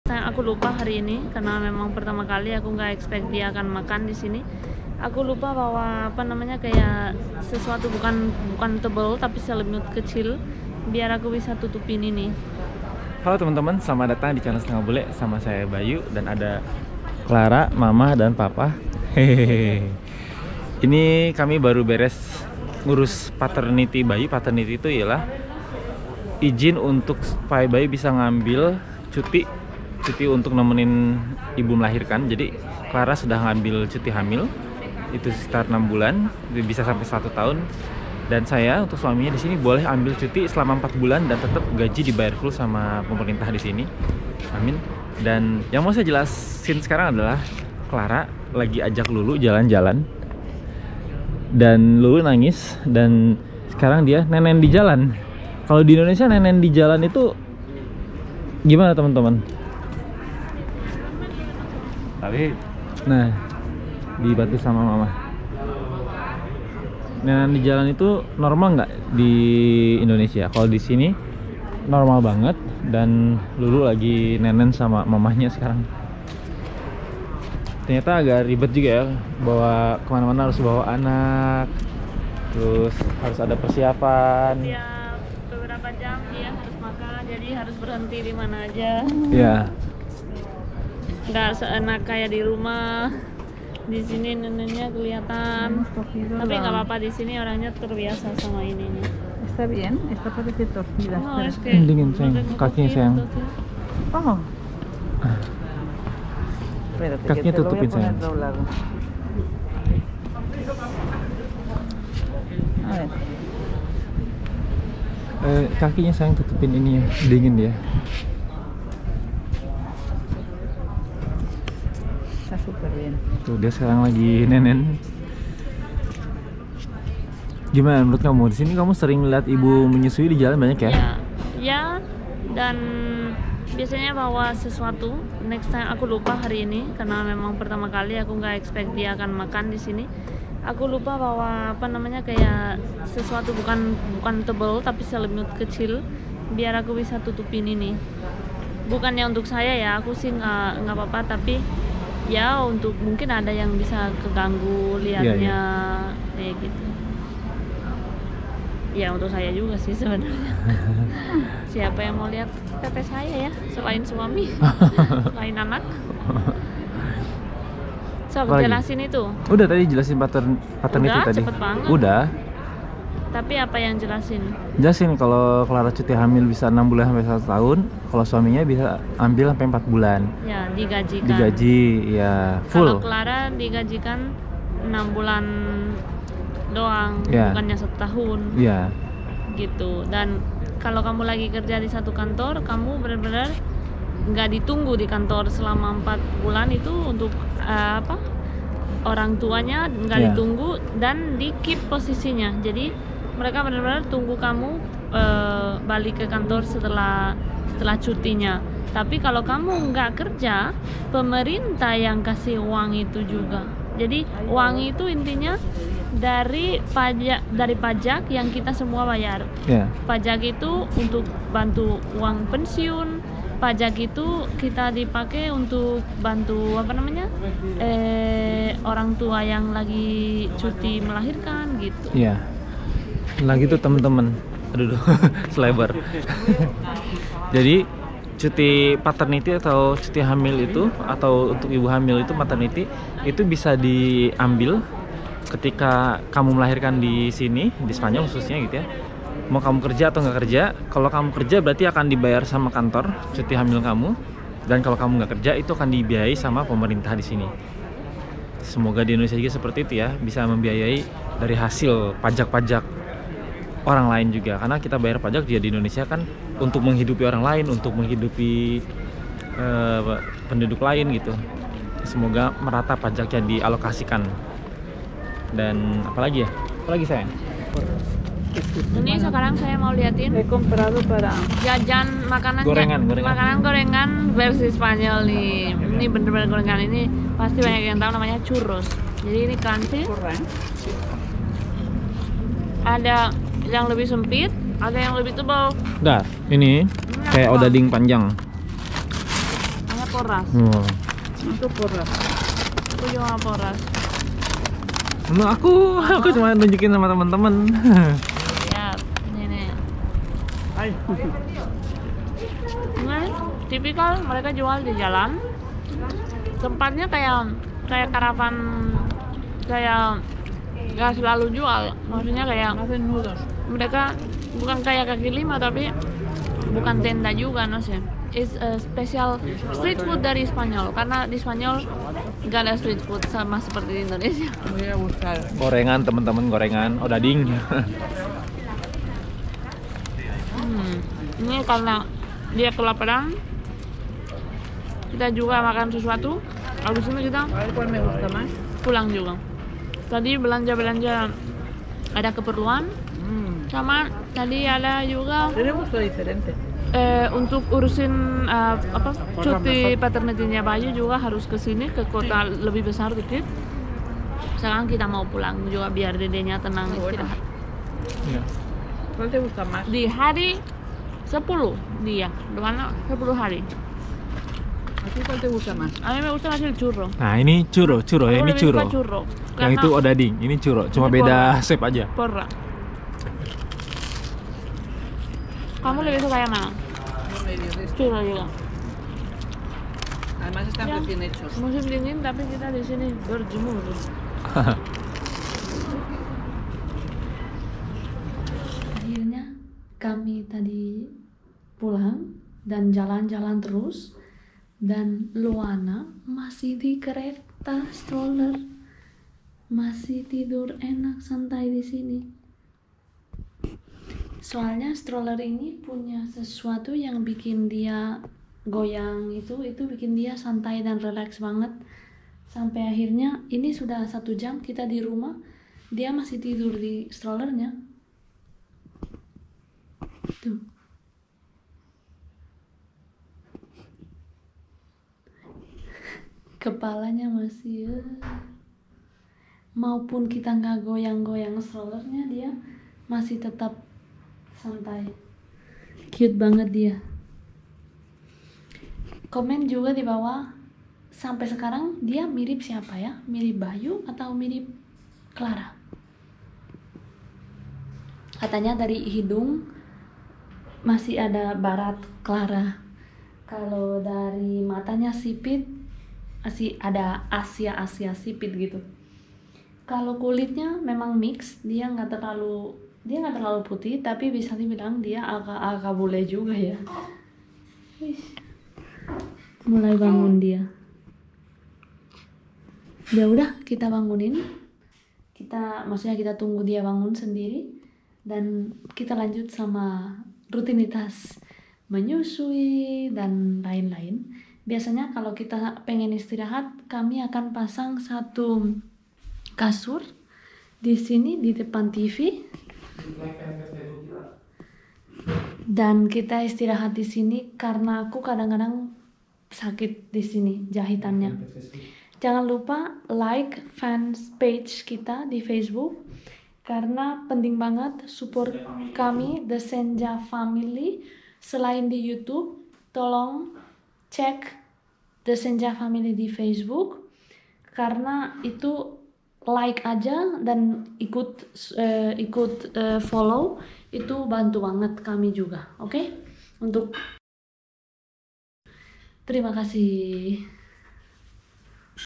Budaya Bule Menyusui Bayinya Dijalan | Sambil bahas cuti hamil
Budaya Bule menyusui bayınya dijalan. Video ini kami bikin karena bertepatan dengan moment Clara menyusui dijalan saat mengantarkan Bayu urus cuti bapak hamil. Menurut teman-teman bagaiamana, Apakah wajar apabila Ibu menyusui bayinya di jalan?